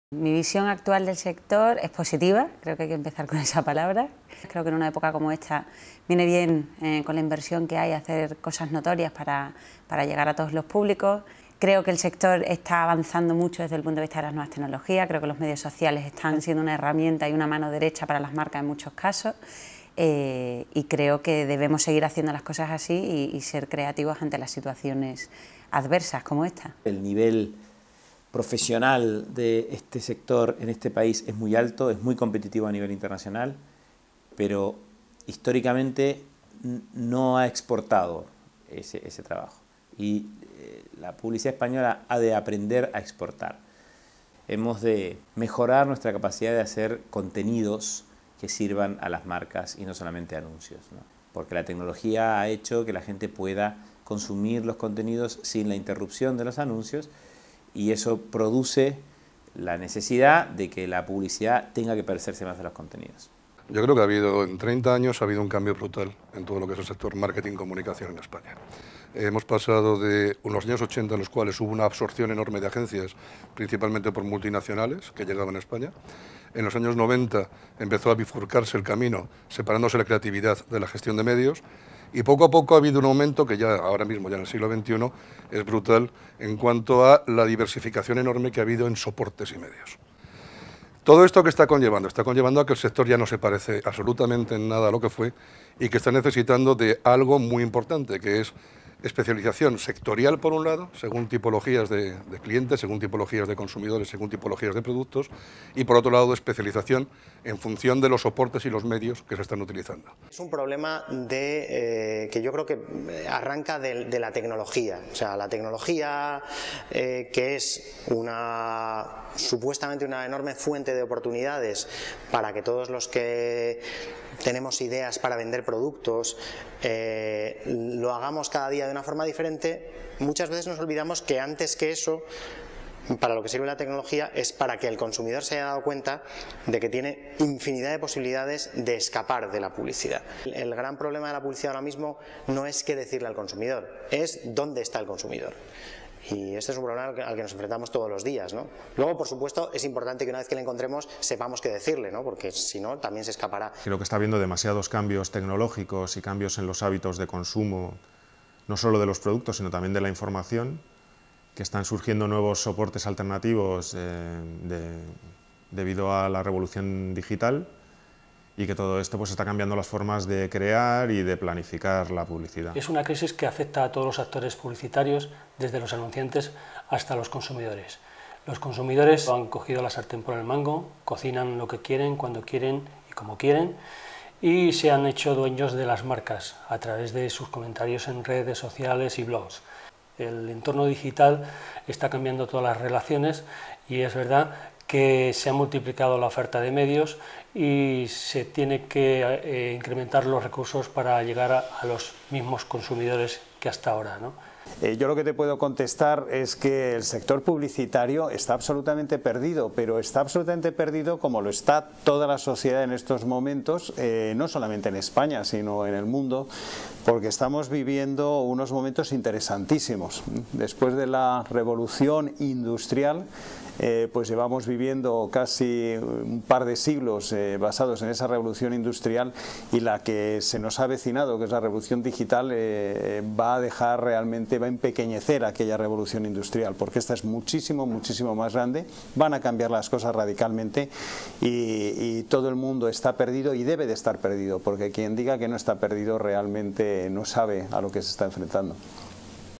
El sector publicitario en España hoy
Algunas de las figuras más relevantes del sector publicitario español nos dan su visión sobre los principales problemas a los que se enfrenta el sector publicitario español hoy para la primera edición de Espacio en Blanco.